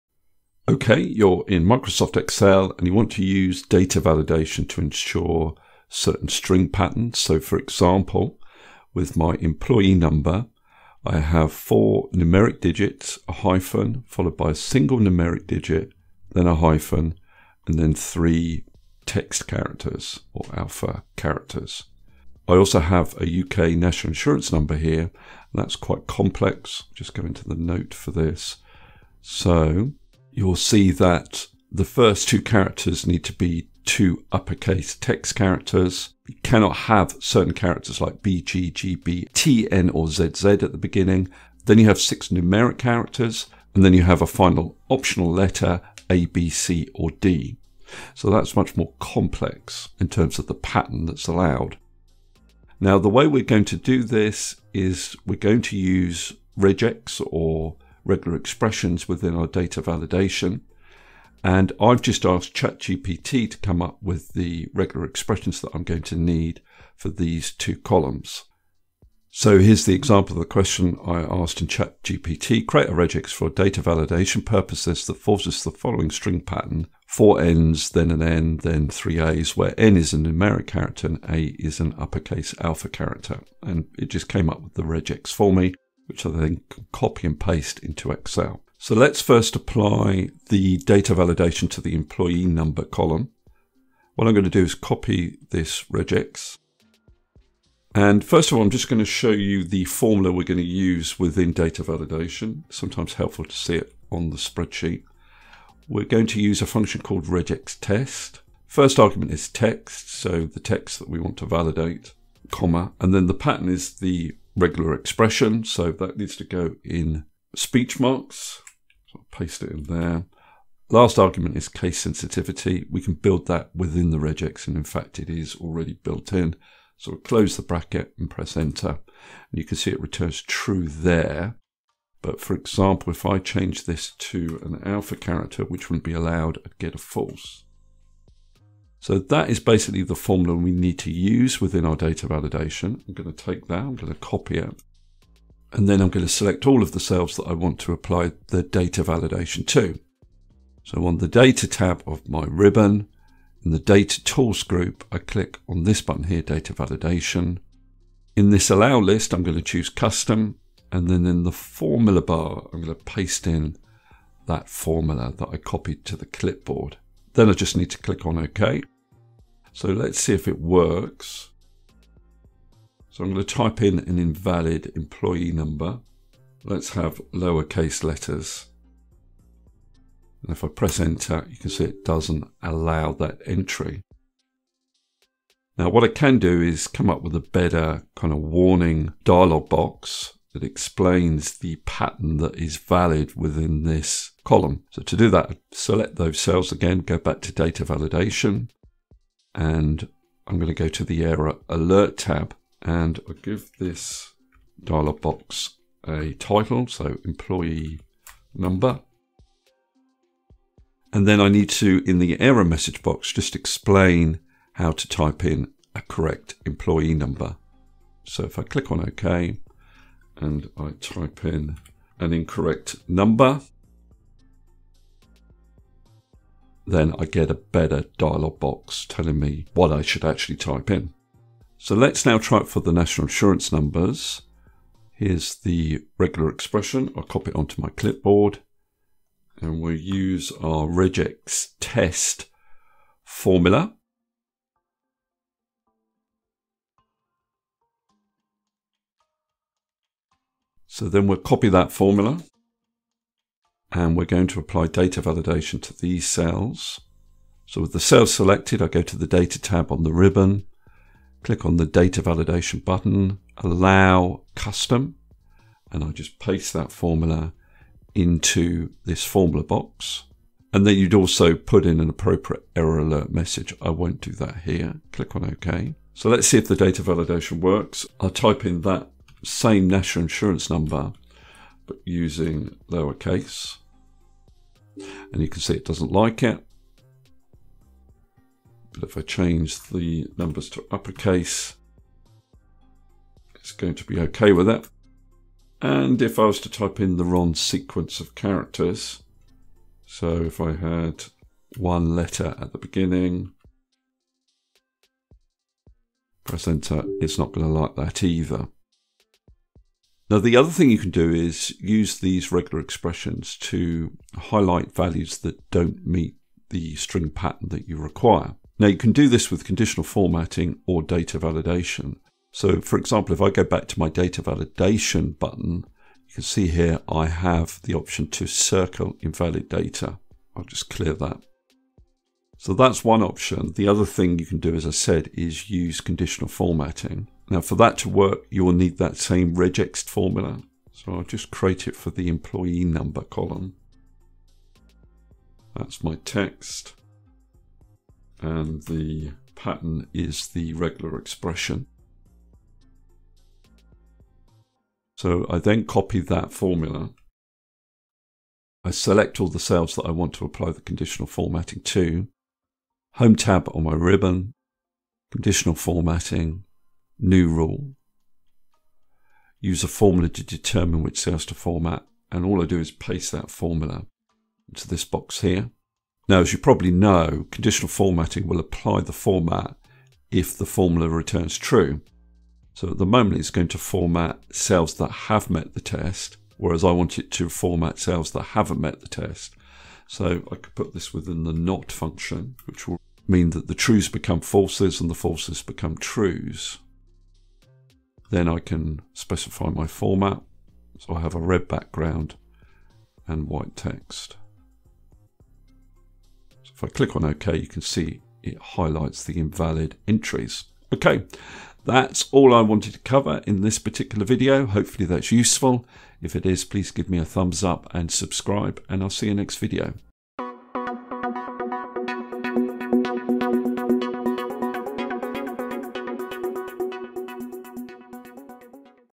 REGEX Data Validation in Excel - String Pattern Data Validation and Conditional Formatting
In this Excel video tutorial I explain how to apply REGEX data validation and conditional formatting.

🕰️ Timestamps
00:00 Introduction
00:52 Using ChatGPT to create regular expressions (REGEX)
01:28 Apply data validation based on the REGEX
05:09 Conditionally format using the REGEX